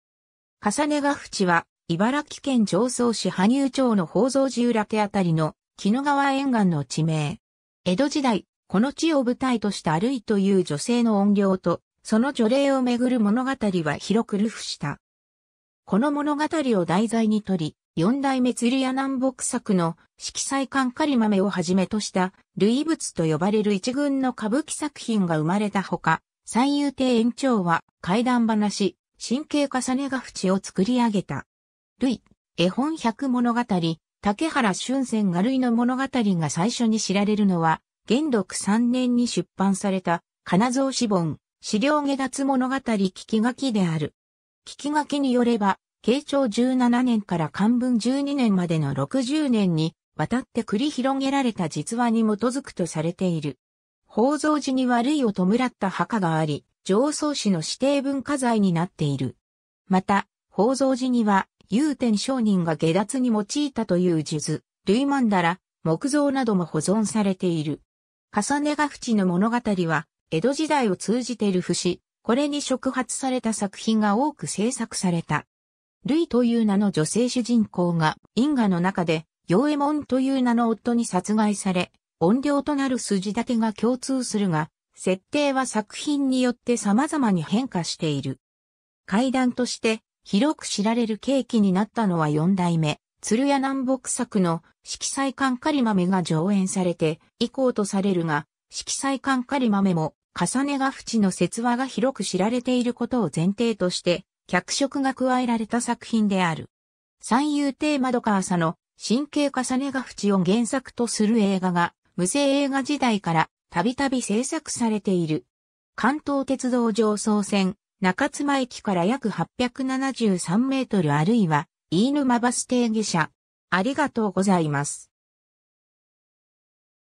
累ヶ淵
累ヶ淵（かさねがふち）は、茨城県常総市羽生町の法蔵寺裏手辺りの鬼怒川沿岸の地名。
江戸時代、この地を舞台とした累（るい、かさね）という女性の怨霊とその除霊をめぐる物語は広く流布した。
 この物語を題材にとり、四代目鶴屋南北作の『色彩間苅豆』（いろもようちょっとかりまめ）をはじめとした累物（かさねもの）と呼ばれる一群の歌舞伎作品がうまれたほか、三遊亭円朝は怪談噺『真景累ヶ淵』を作り上げた。
 累 『絵本百物語』竹原春泉画 累の物語が最初に知られるのは、元禄3年（1690年）に出版された仮名草子本『死霊解脱物語聞書』である。
『聞書』によれば、慶長17年（1612年）から寛文12年（1672年）までの60年にわたって繰り広げられた実話に基づくとされている。
 法蔵寺には累を弔った墓があり、常総市の指定文化財になっている。
また、法蔵寺には祐天上人が解脱に用いたという数珠・累曼陀羅・木像なども保存されている。
 累ヶ淵の物語は江戸時代を通じて流布し、これに触発された作品が多く制作された。
「累」という名の女性主人公が、因果の中で「与右衛門」という名の夫に殺害され、怨霊となる筋立てが共通するが、設定は作品によってさまざまに変化している。
 怪談として広く知られる契機になったのは四代目鶴屋南北作の「色彩間苅豆」（いろもようちょっとかりまめ）が上演されて以降とされるが、「色彩間苅豆」も、累ヶ淵の説話が広く知られていることを前提として脚色が加えられた作品である。
 三遊亭圓朝の『真景累ヶ淵』を原作とする映画が、無声映画時代からたびたび制作されている。
 関東鉄道常総線中妻駅から約873mあるいは飯沼バス停下車